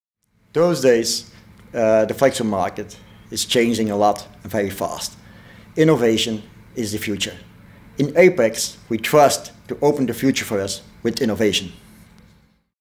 Ludo Peeters Testimonial
Ludo Peeters, Proces Engineer Printing at Smurfit Kappa Turnhout considers Apex International a valuable partner for innovative solutions in the Corrugated industry.